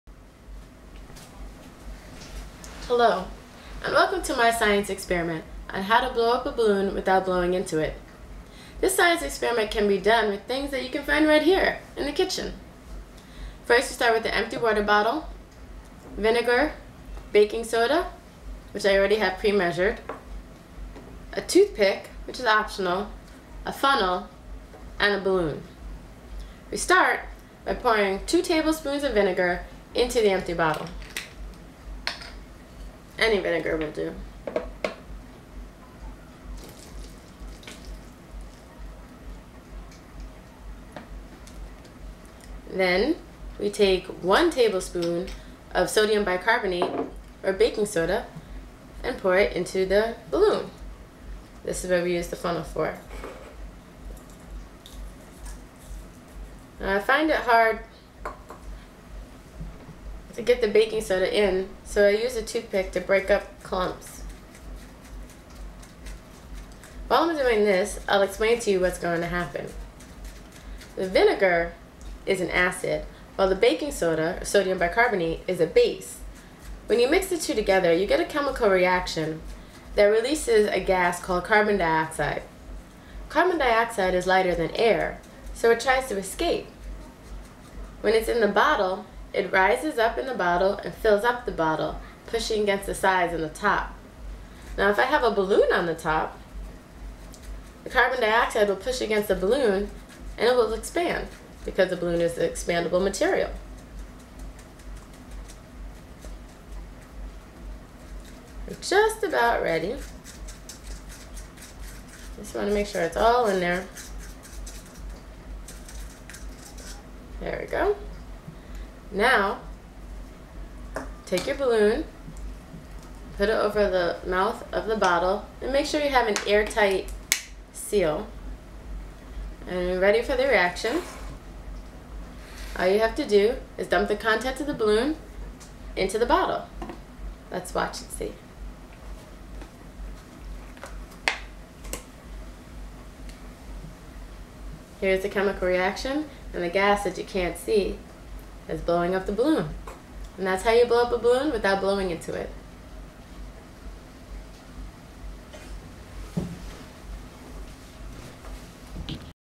Chemical Change: a balloon experimant
Chemical change - How to blow up a balloon without blowing into it